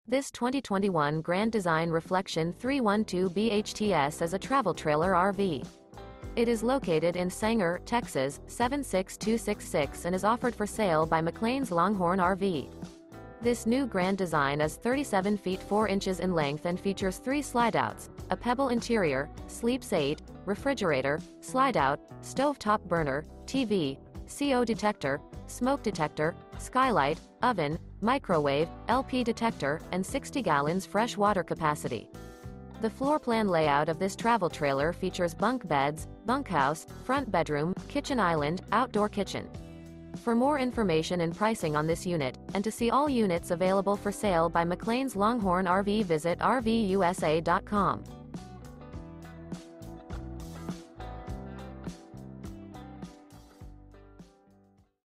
Eye-catching 2021 Grand Design Reflection Travel Trailer RV For Sale in Sanger, TX | RVUSA.com
This 2021 Grand Design Reflection 312BHTS is a Travel Trailer RV. It is located in Sanger, TX 76266 and is offered for sale by McClain's Longhorn RV. This New Grand Design Is 37' 4" in length and features 3 slideouts, a Pebble interior, sleeps 8, Skylight, Stove Top Burner, TV, Smoke Detector, Microwave, LP Detector, CO Detector, Air Conditioning, Refrigerator, Oven, and 60 gal fresh water capacity. The floorplan layout of this Travel Trailer features Bunk Beds, Bunkhouse, Front Bedroom, Kitchen Island, Outdoor Kitchen.